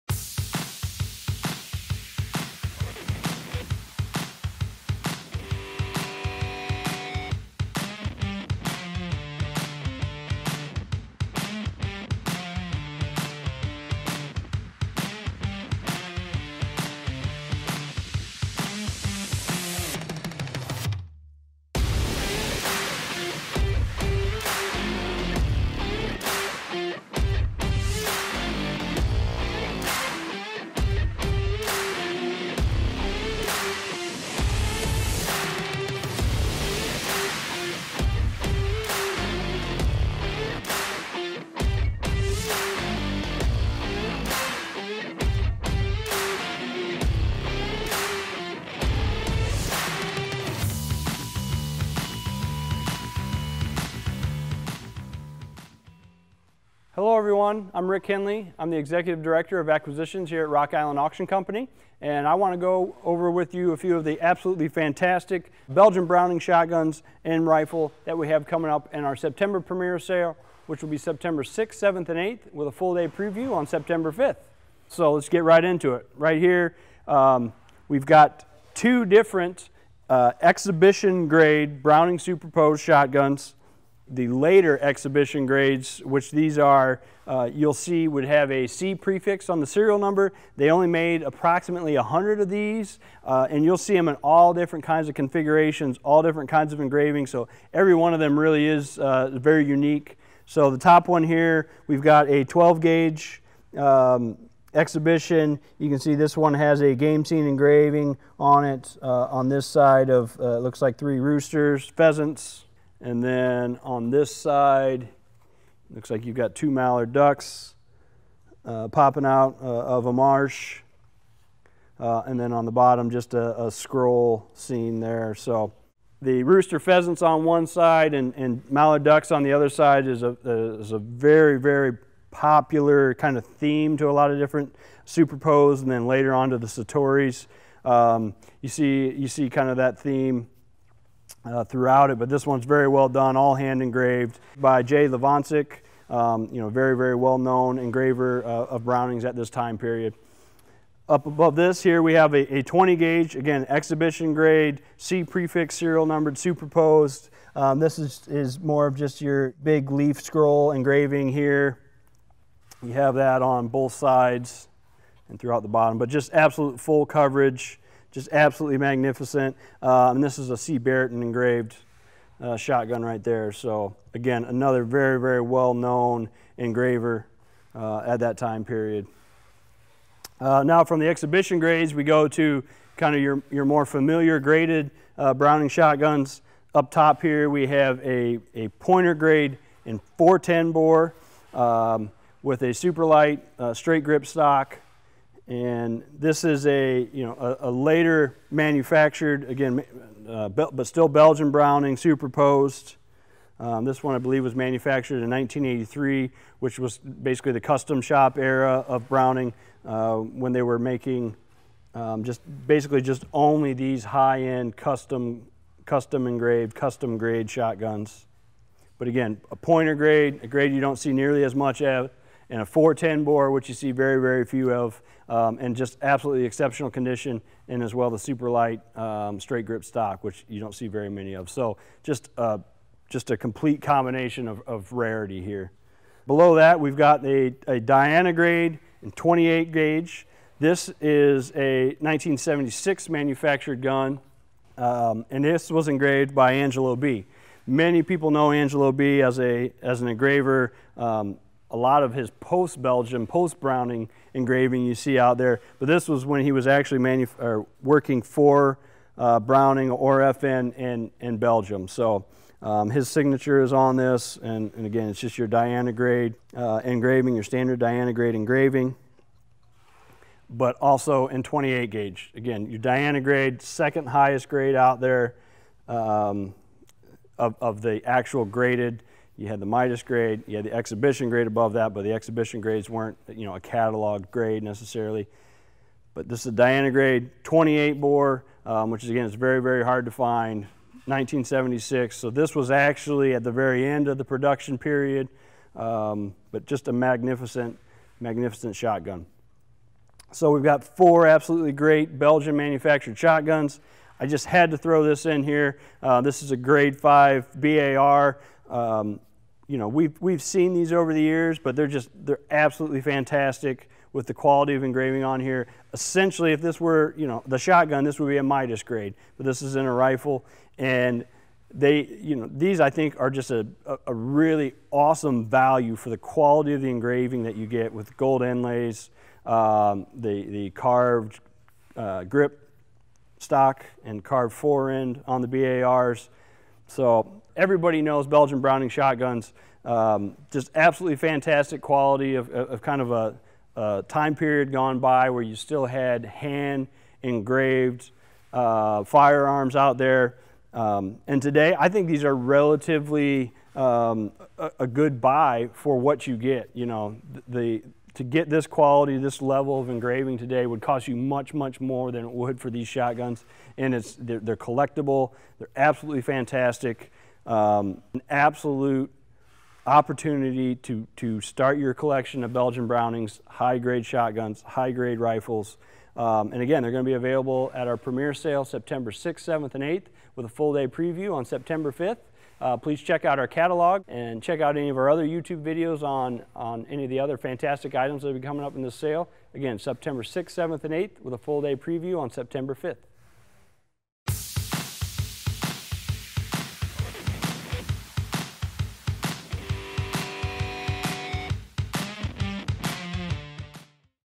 High Grade Belgian Brownings
Browning is known for its finely detailed and beautifully crafted hunting firearms. Gorgeously engraved game scenes and delicate gold inlays decorate these stunning Belgian Brownings. From two highly desirable exhibition grade beauties to a collection of other wonderful Browning shotguns, the firearms before you are examples of high art hunting guns. We even threw in a magnificent Browning rifle for good measure.